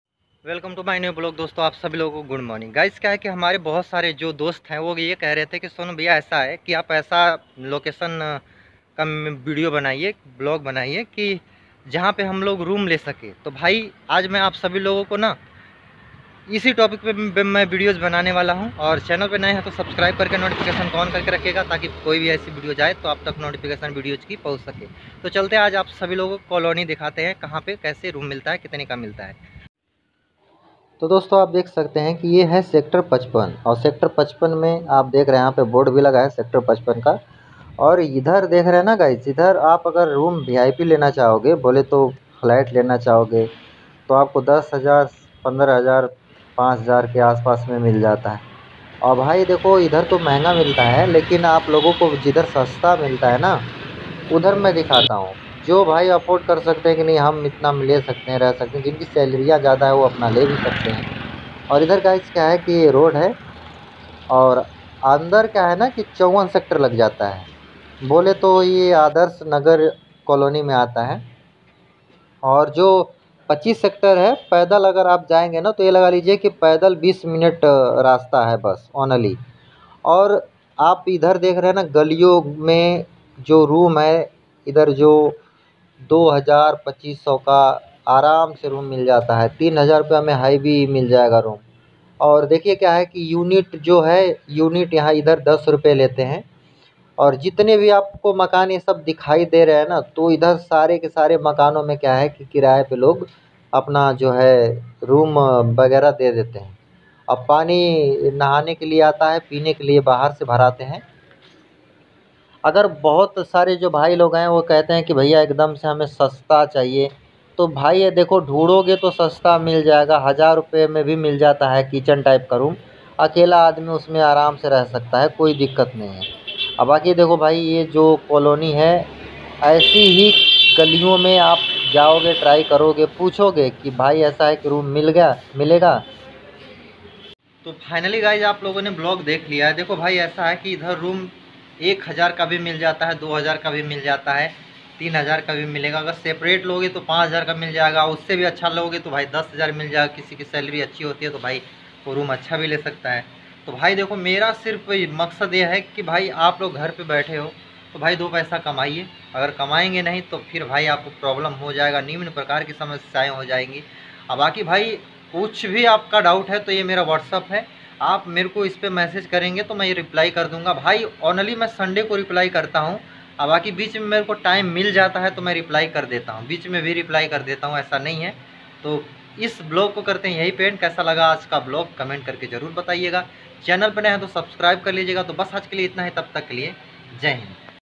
किराए पर रूम 🏣फरीदाबाद ,हरियाणा में इधर मिलता है! Faridabad, Haryana Vlogs
Hello Friends Es Vlogs Me Kaha Par Rent Par Room Milta Hain Aap Logo Ko Dikhaya Hain.

Address.. Sector 55/54 Adarsh Nagar

Faridabad. Haryana